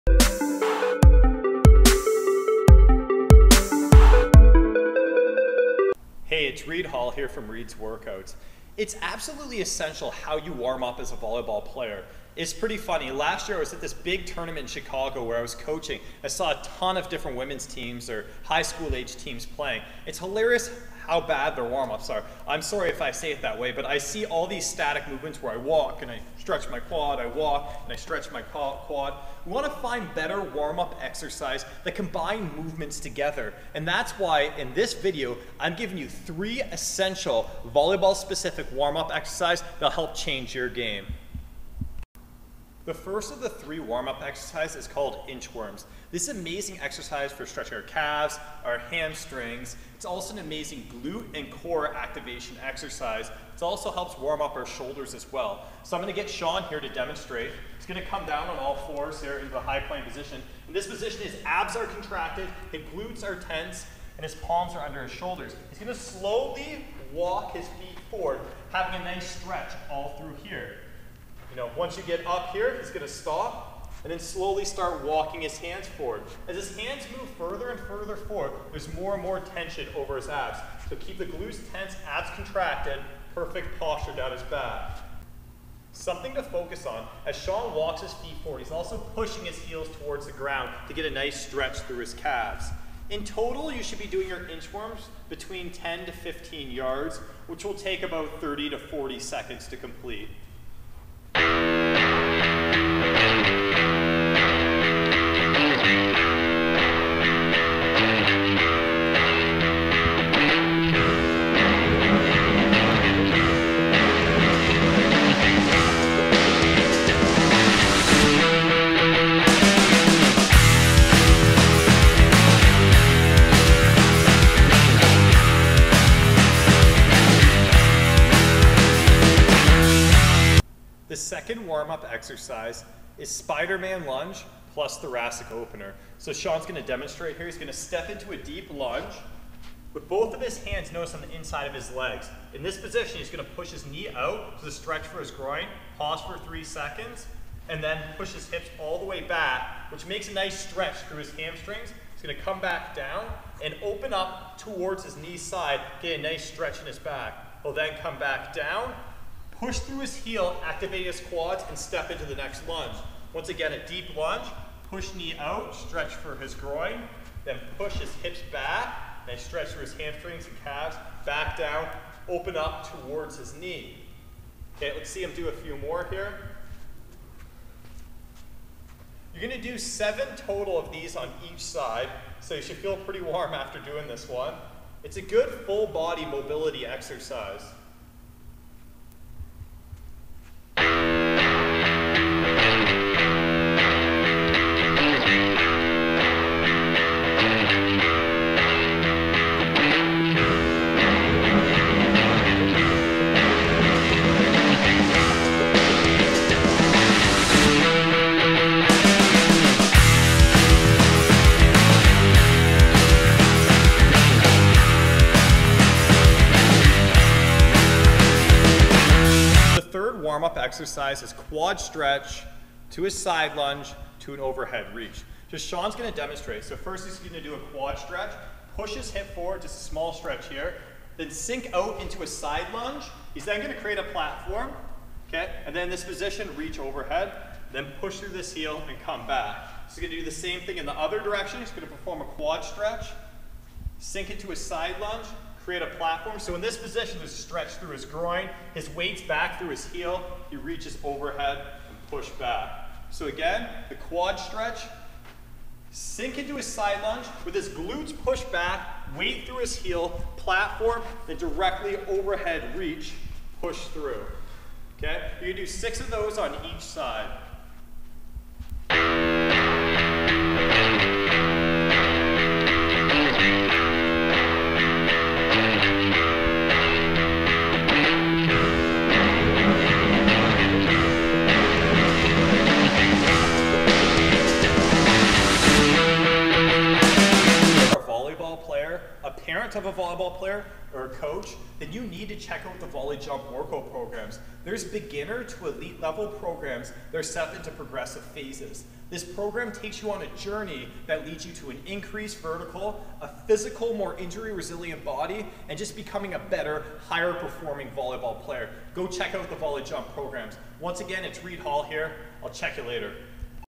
3 Essesntial Warm Up Exercises for Volleyball Players
Get INSTANT ACCESS to VolleyJump Volleyball Workout Programs for just $1
Every volleyball player has played a match where early on in the game they felt overly stiff, low on energy, and in pain whenever they would do something explosive. This is common when you do not have a proper warm up before you compete.

Why give the team your competing against a competitive edge by not performing a high quality warm up yourself?

A proper warm up will not only have you performing better early on in the match but will also significantly reduce your injury rate.

A significant amount of volleyball related injuries could be prevented by properly warming up your body before every practice, match, and workout.

Your warm up exercises should help activate and deliver you more blood flow to your muscles, help keep your joints functioning properly, and prepare your body to jump higher, hit harder, and move faster.

Here are 3 high quality warm up exercises that you should be performing…